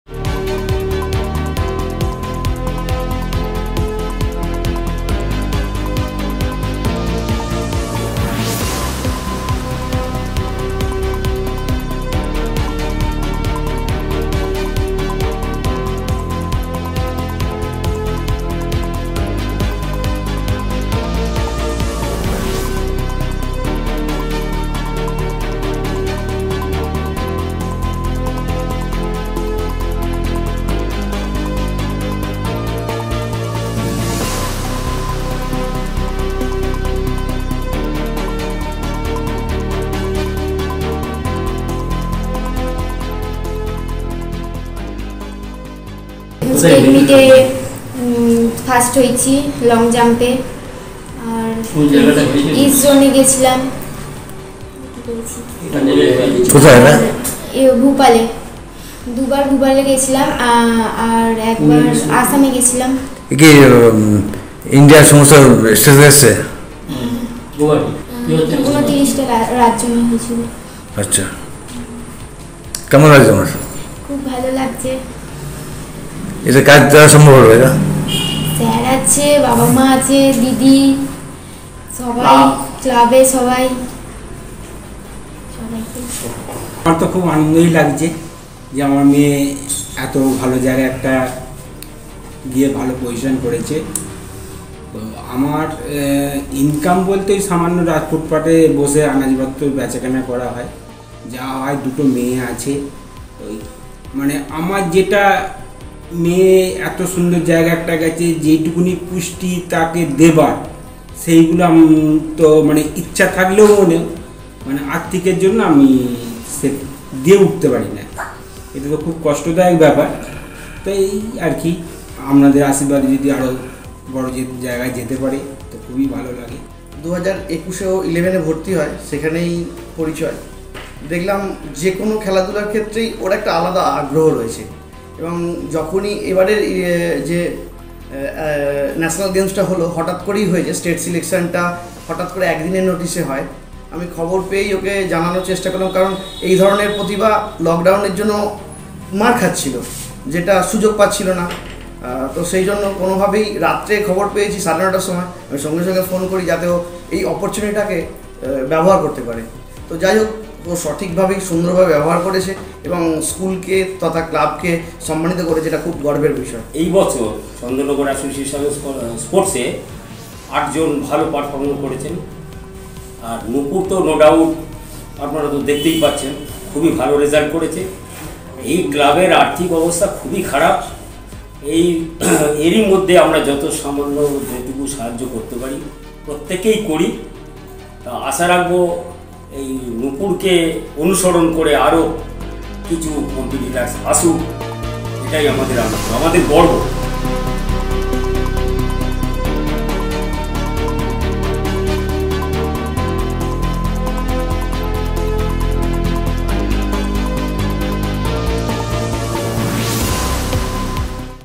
District News: Hooghly Girl wins Gold Medal at National Games | Sangbad Pratidin
Hooghly Girl wins Gold Medal at National Games.

Gold Medal Winner Nupur Pandey's father, teacher & coach are happy with her gold win despite family hardships. Her father always encouraged her daughter. Nupur said she got lots of help from her school teachers & coaches. His future target is the Asian Games.